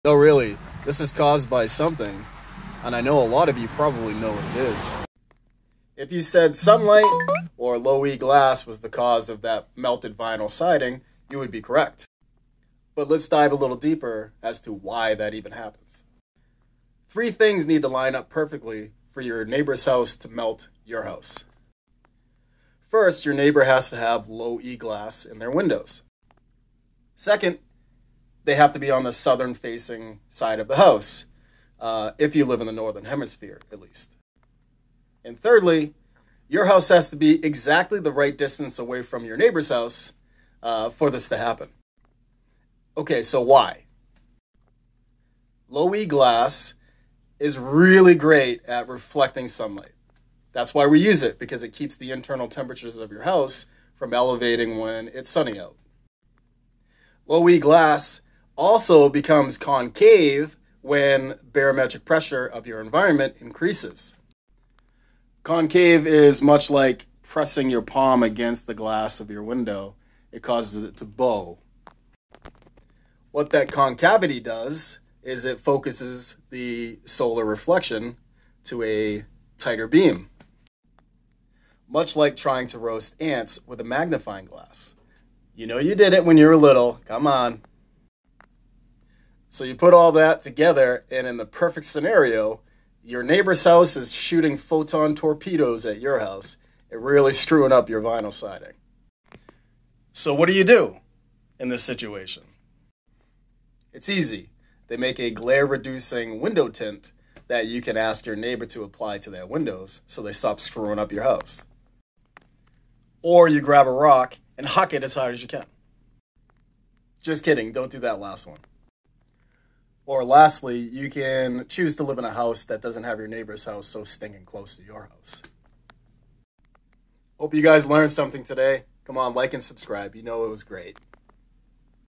your neighbor's low E glass windows could be melting your house
low E glass is great at reflecting sunlight, but in the right scenario your neighbor's Windows could be melting your vinyl siding.